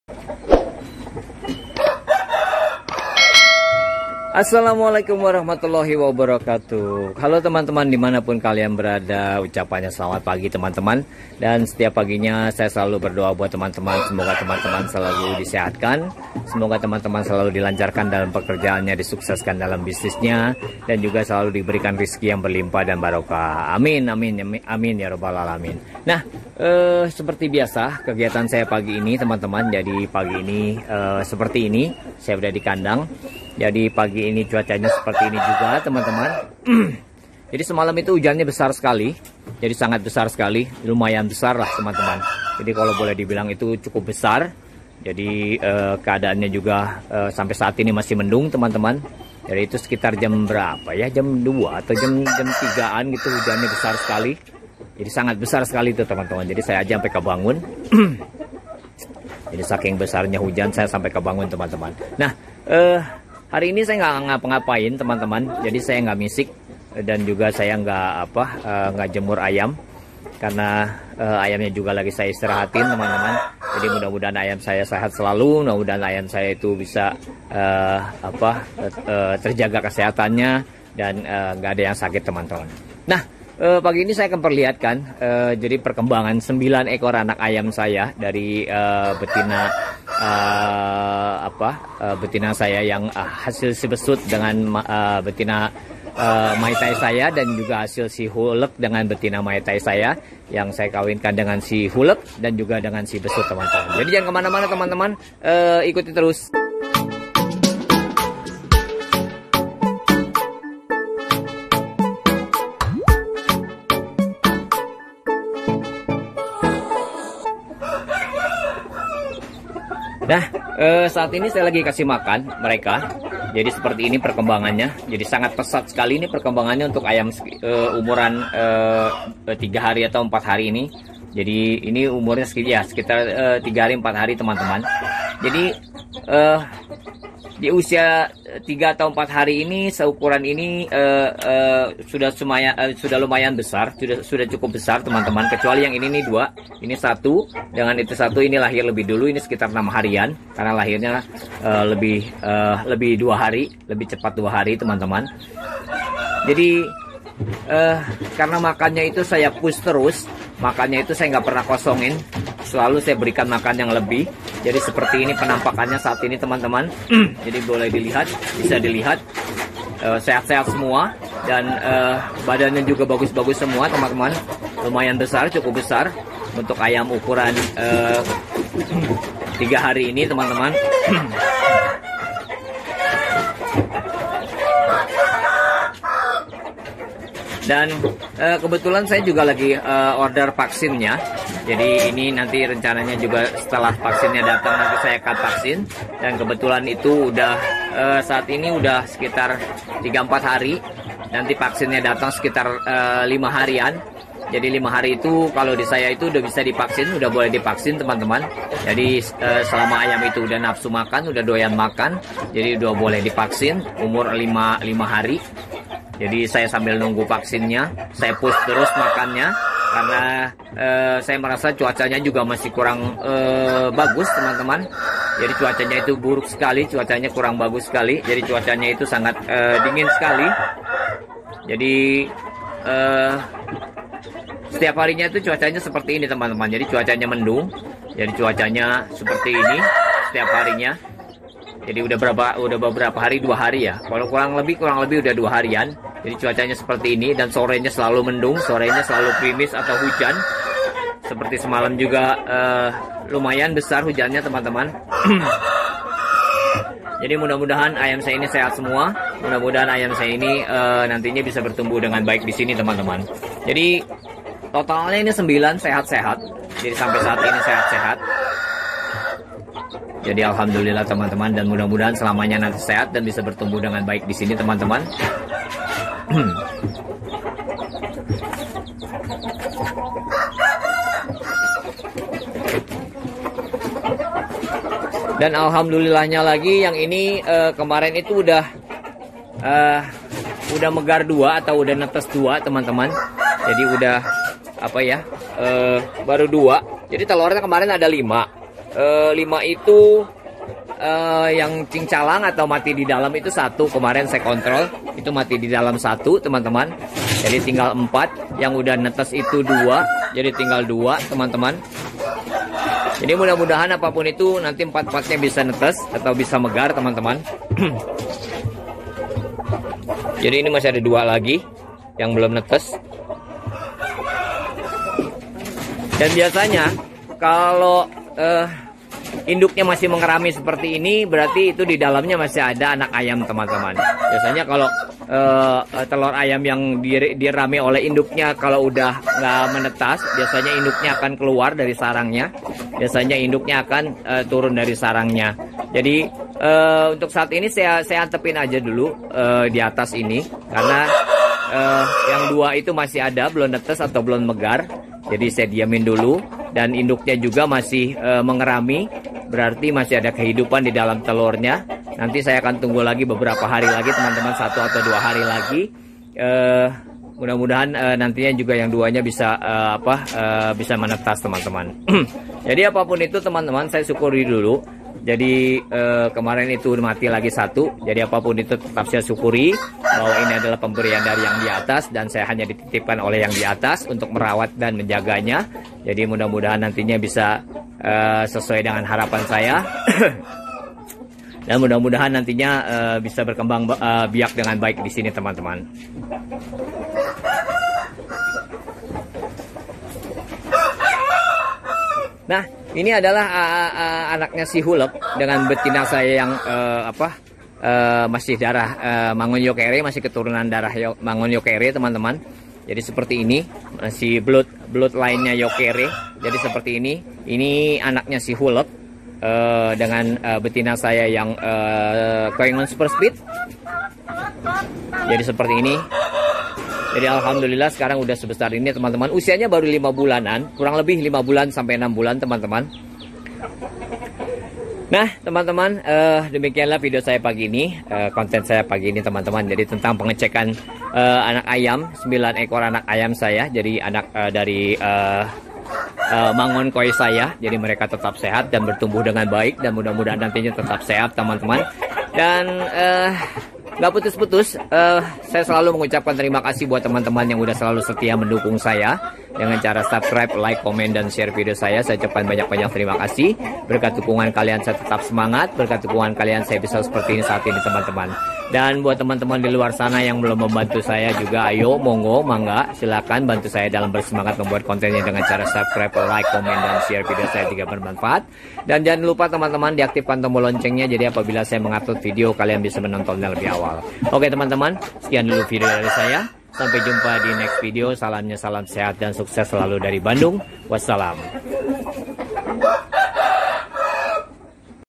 DOC Ayam Trah Juara
Selamat pagi semua nya, konen kali ini tentang pertumbuhan DOC yang cepat besar di kandang